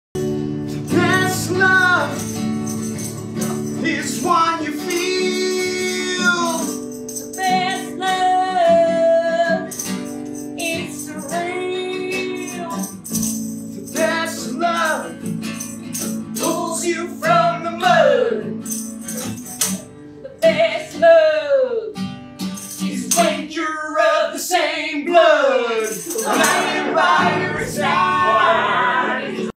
Wait run that back!
As seen in I Mostly Blame Myself at The Players Theatre in NYC.

Sketch title: The Wilkins Siblings
Writer: Nick Shanman
Co-writer: Jackson Hinden
Starring: Jackson Hinden, Sean Mana, Allison McHugh, and Kaya Simmons
DP: Jake Mitchell
Lighting Designer: Patrick Moriarty
Sound Engineer: Austin Rick